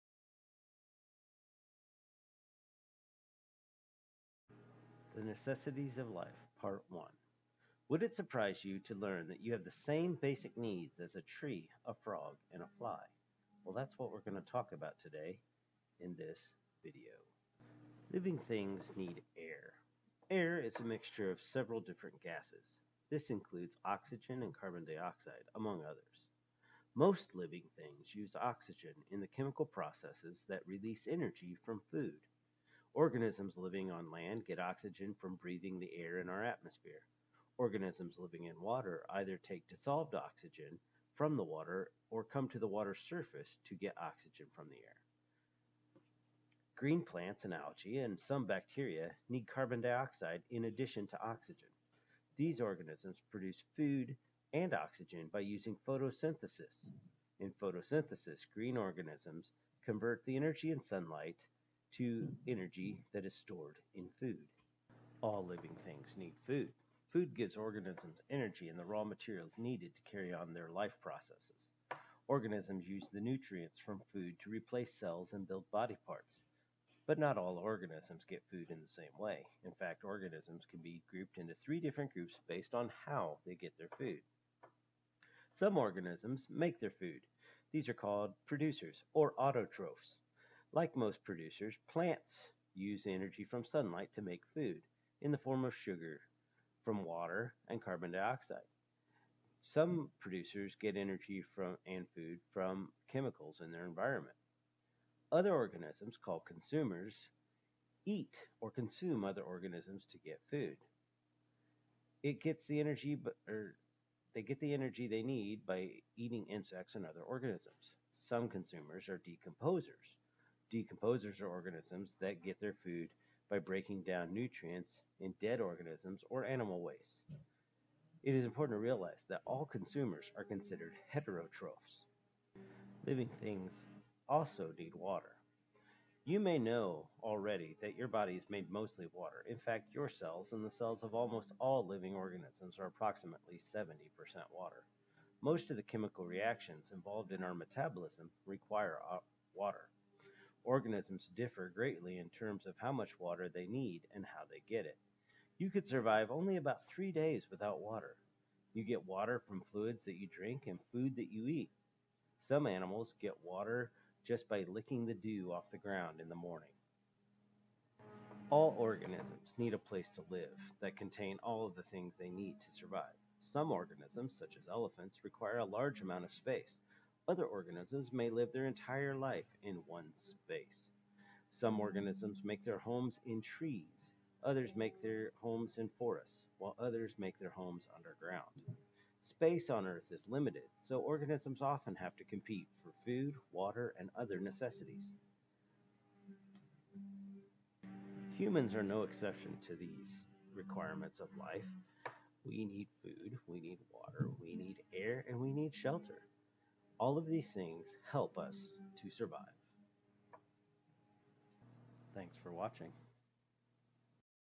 The Necessities of Life Part 1
This week's lessons focus on the Necessities of Life and what living organisms need to survive.